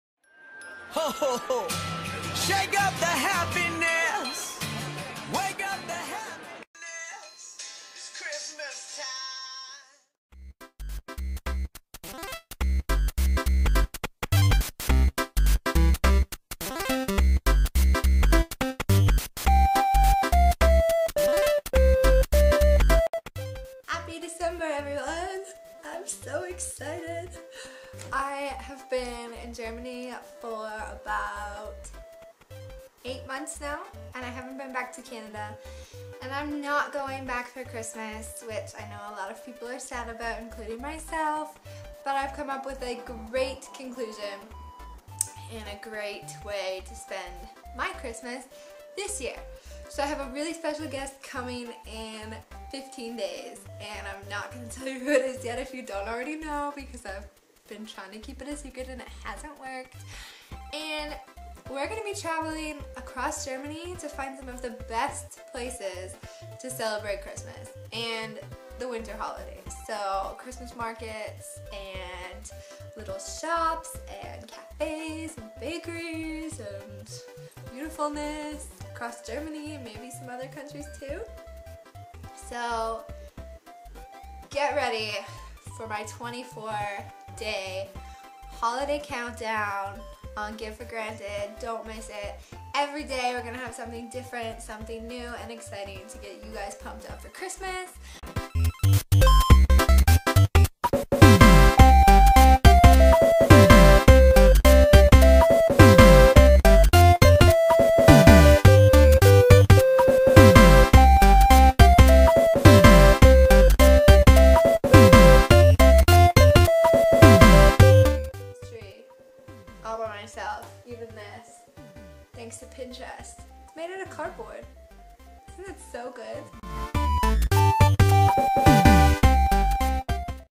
THE HOLIDAYCOUNTDOWN HAS BEGUN
What I've been waiting for at least for the passed few months... the holidaycountdown!

I will be blasting you with new and exciting updates, Christmas Market tours, tips and tricks for the holidays and whole BUNCH MORE!

I will try and upload a couple videos a week minimum so you can really get into the spirit of things. If you haven't been to a Christmas Market before, you don't know what you're missing!

►►►►►►► MORE FUN ☼ GIVE FOR GRANTED ◄◄◄◄◄◄◄

Follow the Give for Granted blog!